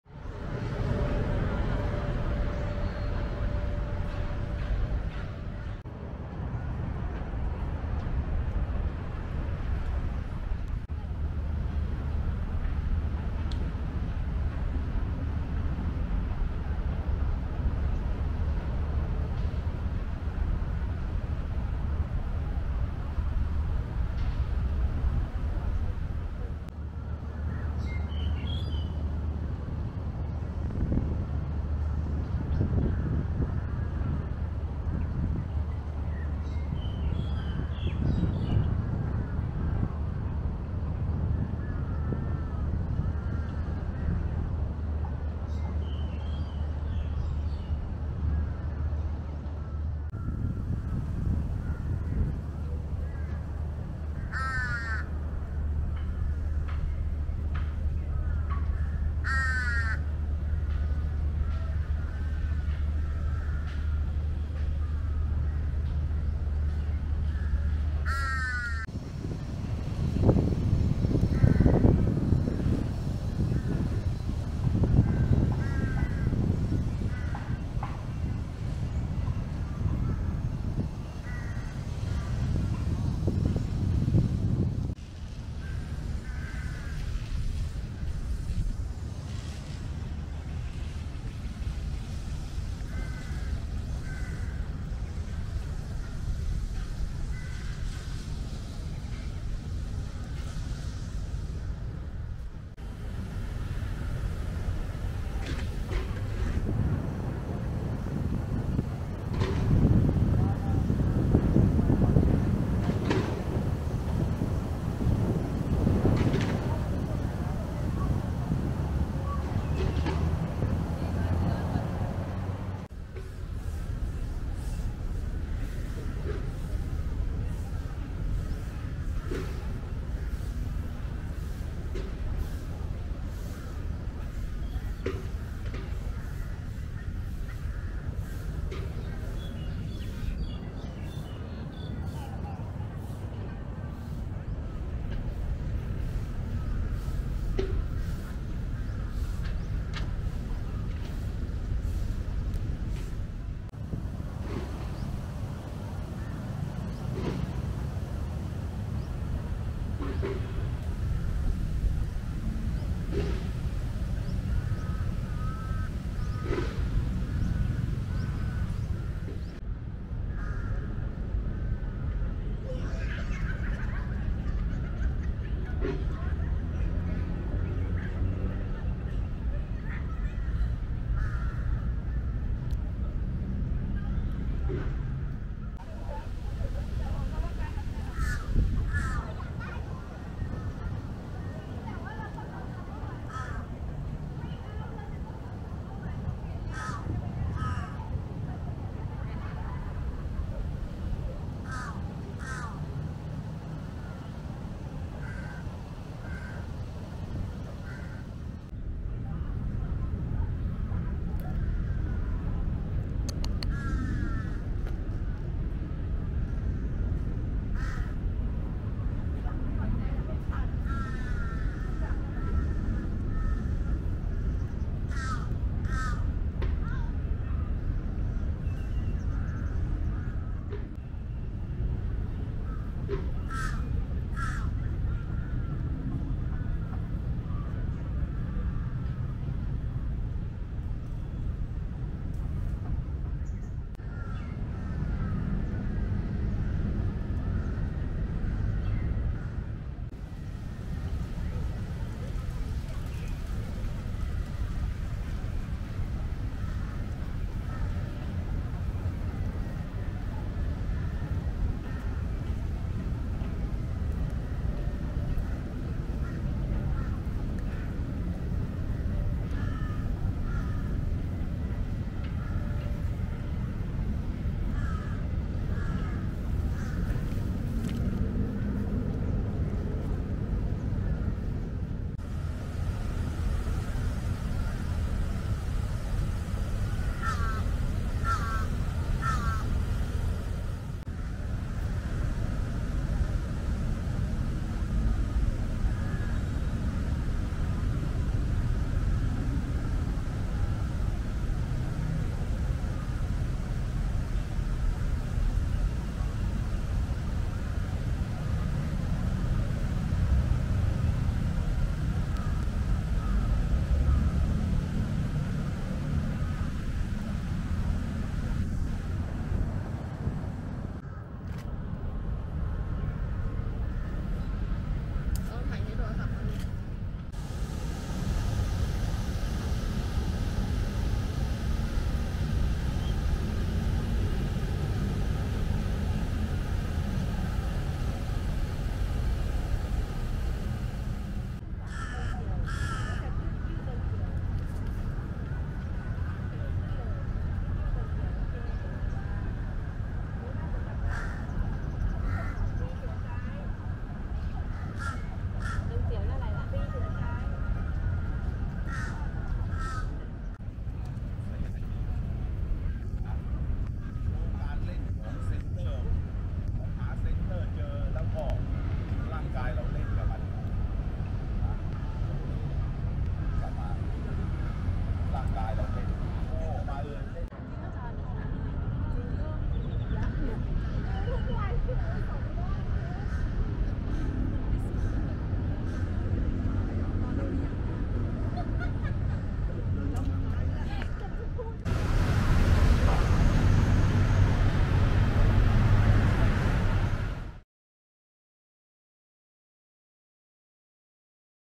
Bangkok Lumpini park 18 aout 2023
Lumpini park
situé au centre de Bangkok
On peut y accéder par le BRT station Silom
ou le BTS station Saladaeng
Belle ballade en vue.
Conseillée le matin ou en fin de journée, il peut faire chaud
Vélo, jogging, tai chi sont au programme.
De nombreux varans, non agressifs se pélassent au bord de l'eau.
On peut s'y restaurer